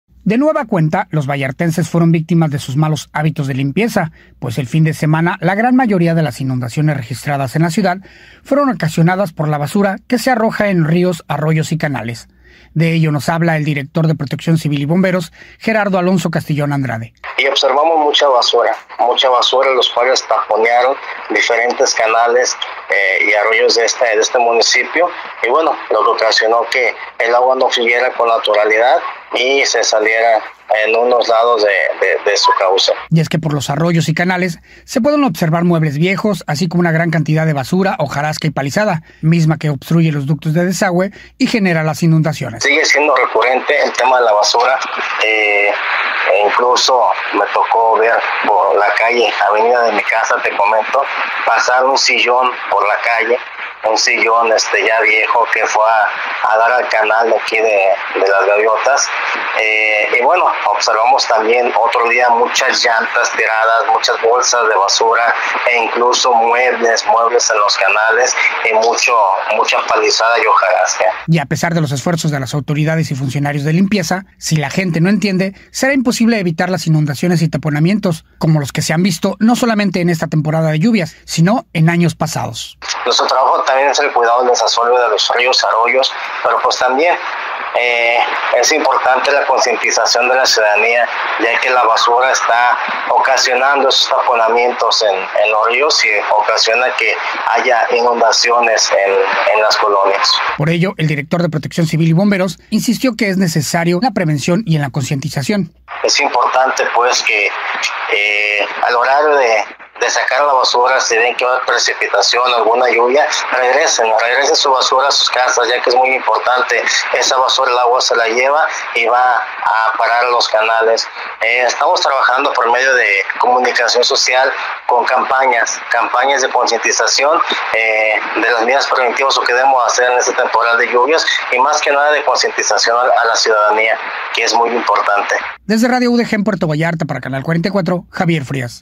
Basura en Puerto Vallarta ocasionó la mayoría de las inundaciones registradas el fin de semana.
Puerto Vallarta vuelve a ser víctima de los malos hábitos de limpieza de sus habitantes, pues la basura ocasionó la mayoría de las inundaciones registradas el fin de semana